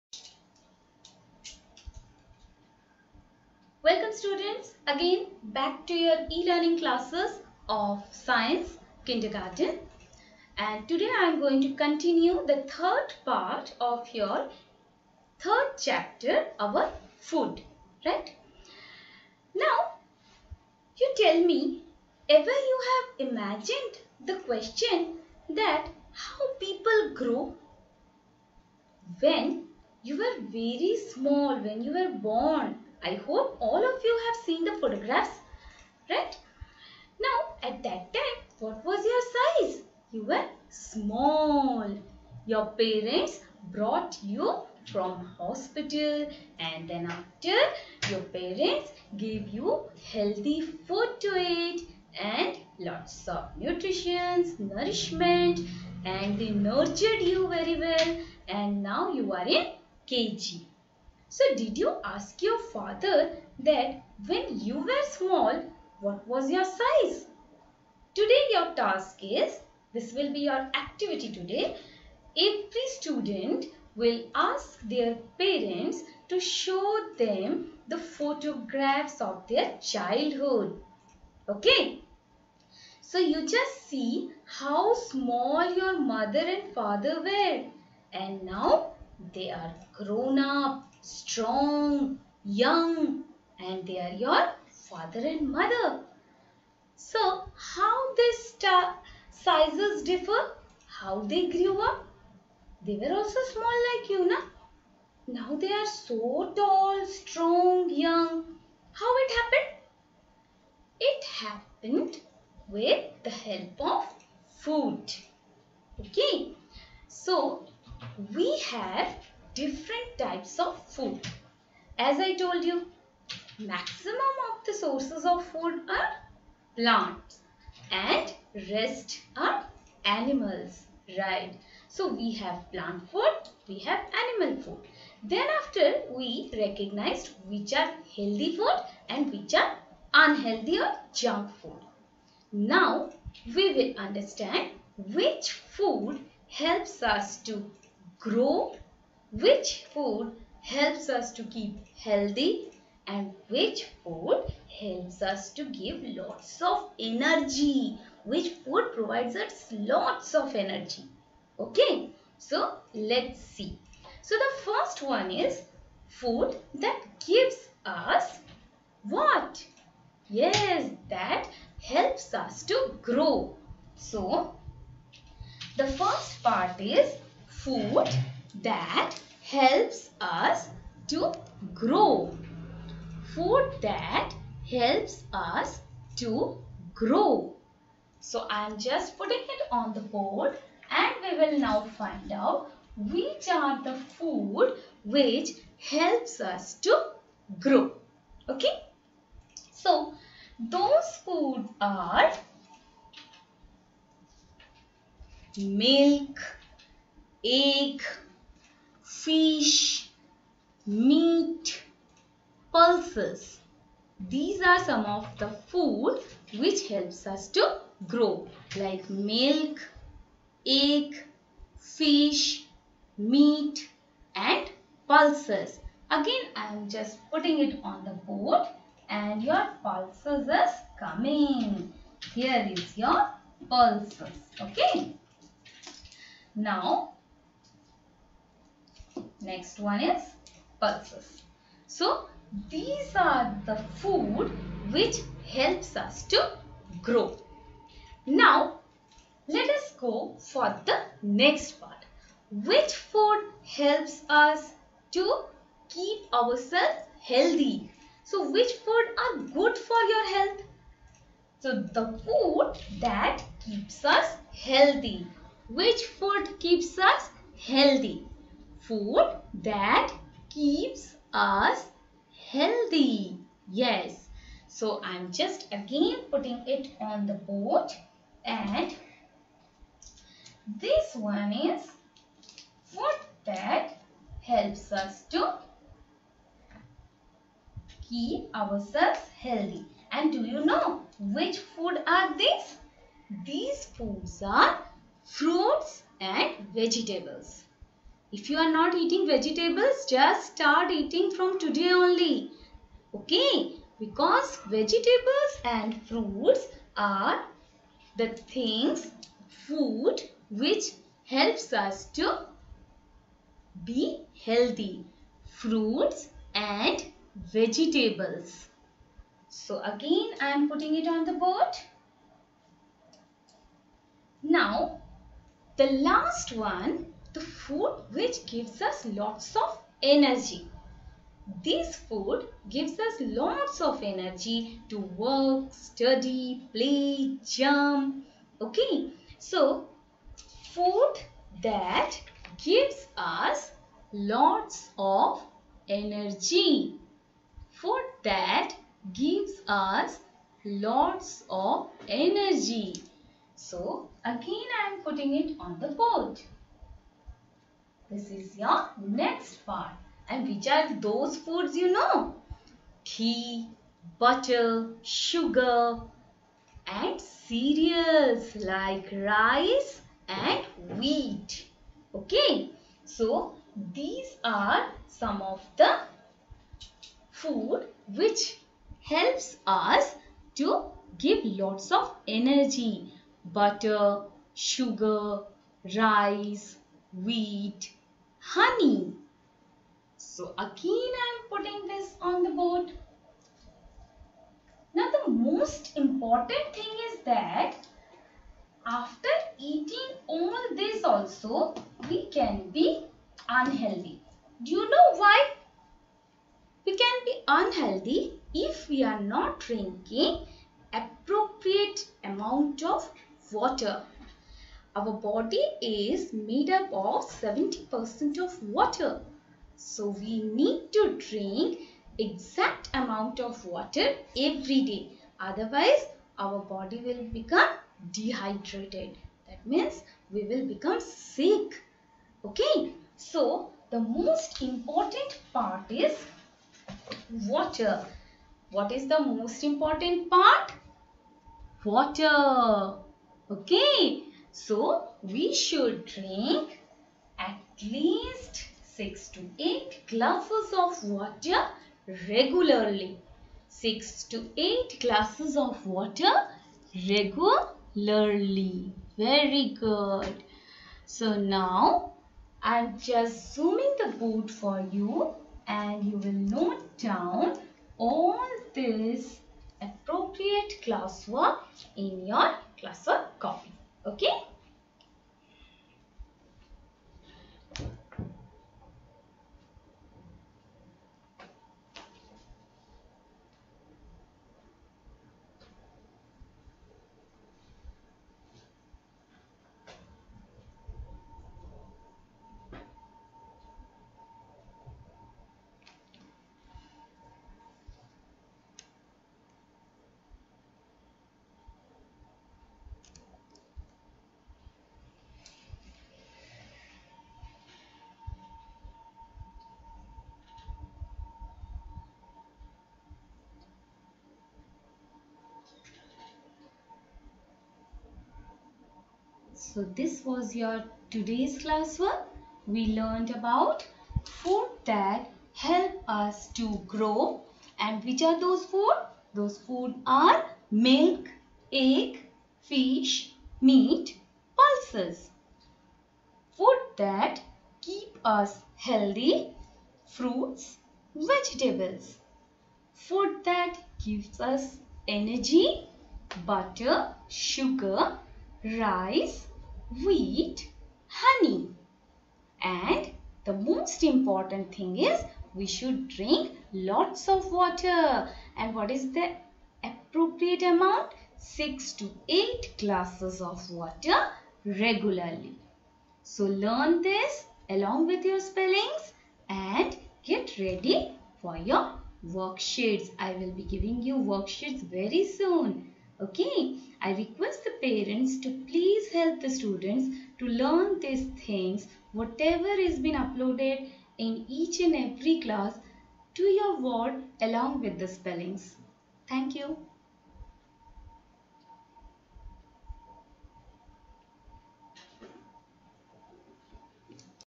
Class KG Science(Topic : OUR FOOD)
Class KG
Subject : Science
Topic :  OUR FOOD
Teacher : Sunita Marik Malakar